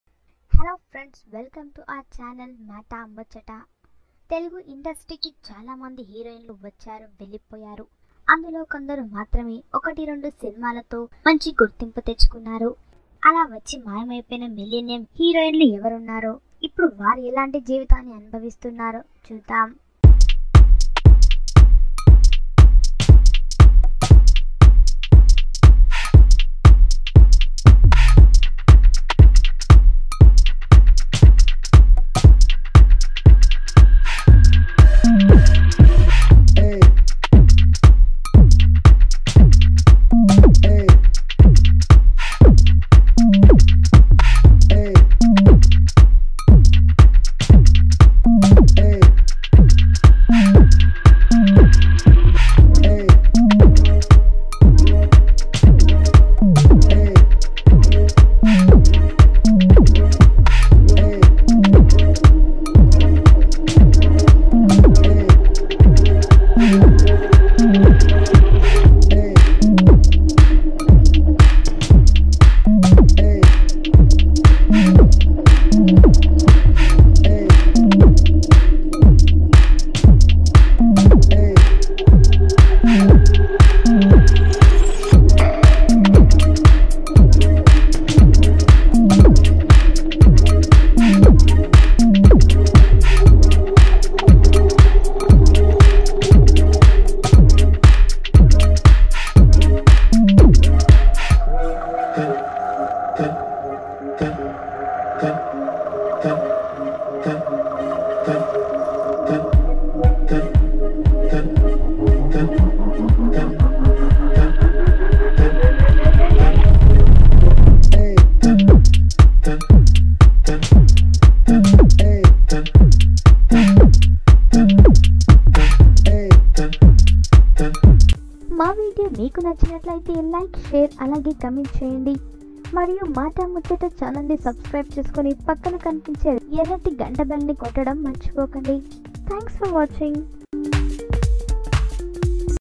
సడన్ గా టాలీవుడ్ నుంచి మాయమైన ఈ హీరోయిన్స్ ఇప్పుడు ఎలాంటి స్థితిలో ఉన్నారో చూసారా? Tollywood Actors
Tollywood Heroines Far Away from Movie Industry..!! Here is the Reason